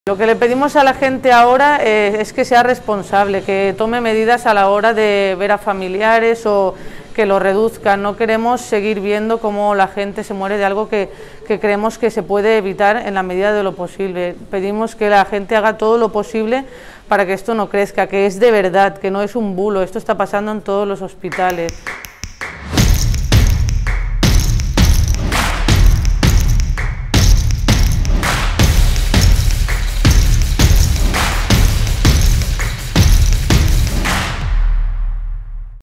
Ajuda'ns a cuidar-te, fem equip contra el virus | Marina, Auxiliar Sanitari
Com a sanitària, quin missatge enviaries a la població en aquesta segona onada de la COVID? Respon la Marina, auxiliar sanitària del Clínic.